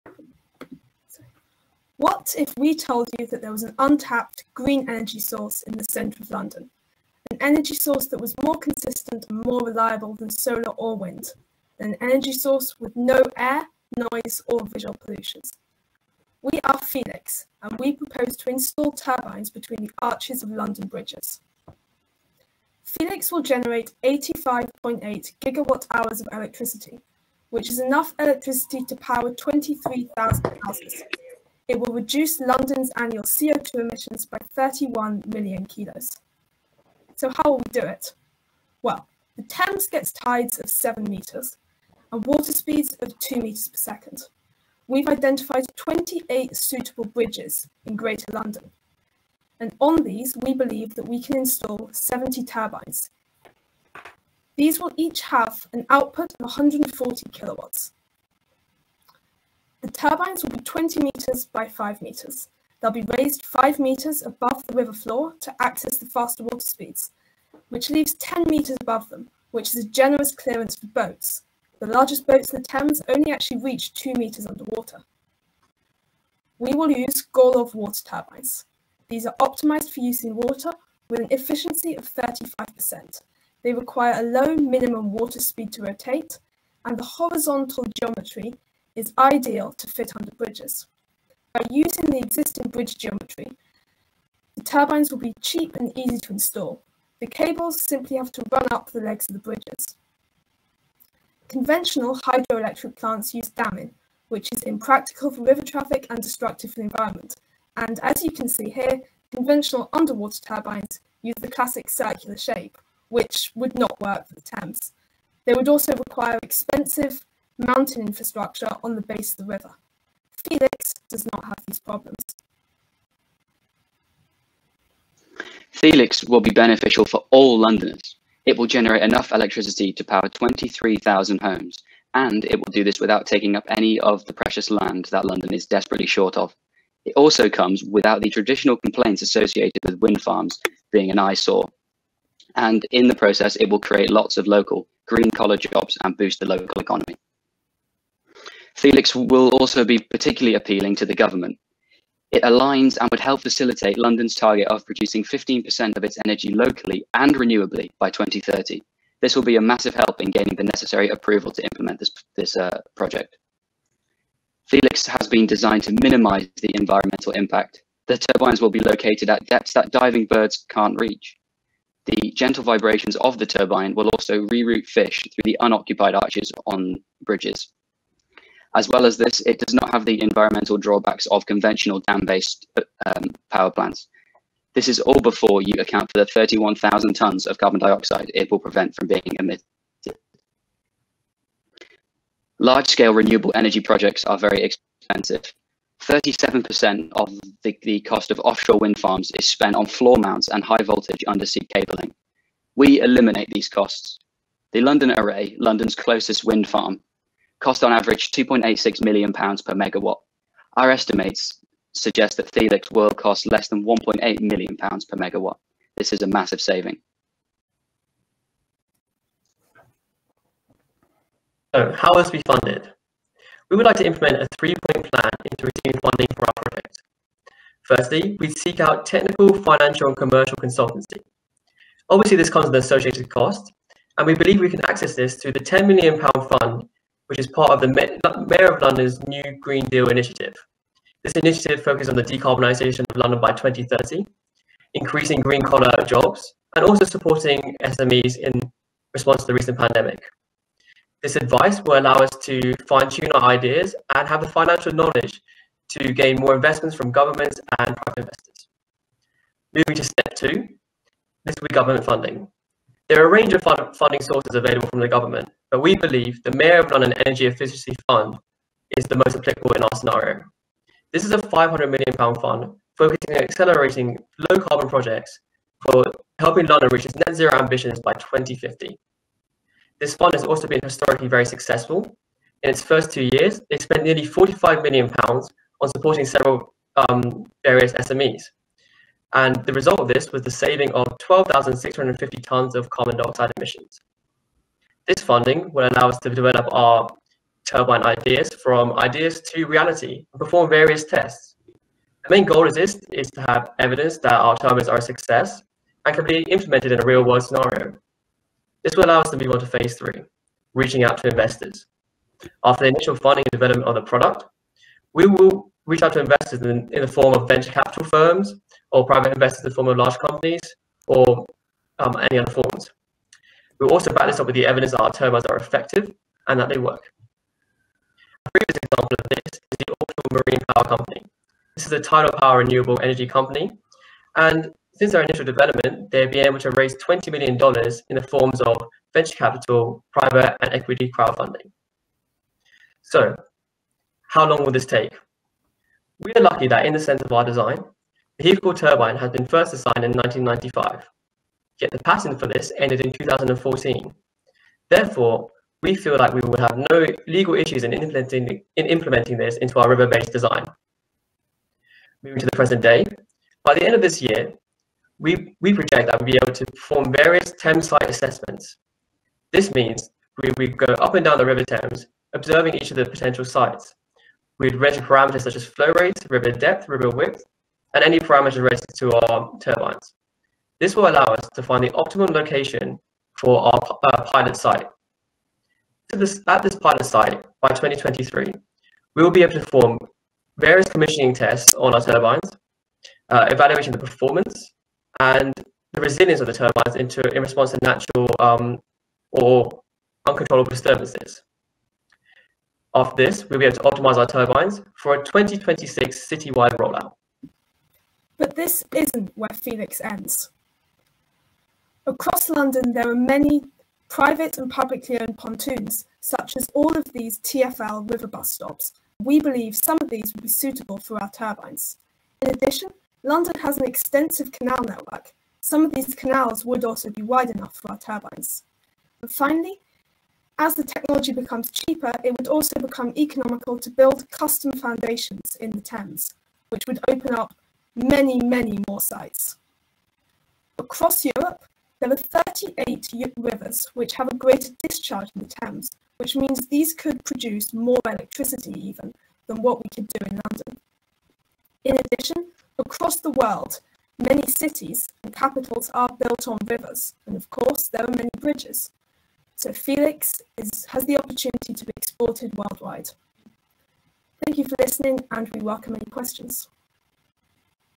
2021 Energy Idea Challenge Winners' Presentation: THelix - Installing turbines in the Thames
Earlier this year, we established the Octopus Centre for Net Zero (OCNZ) - a centre for independent research, engagement and open technology – harnessing the power of the tech sector to find faster, cheaper, and fairer routes to Net Zero. The Octopus Centre for Net Zero put the capital's brightest young minds to the test at the very first London Energy Idea Challenge  – a weekend long hackathon. Thelix, with their proposal to harness energy from the Thames were our winners!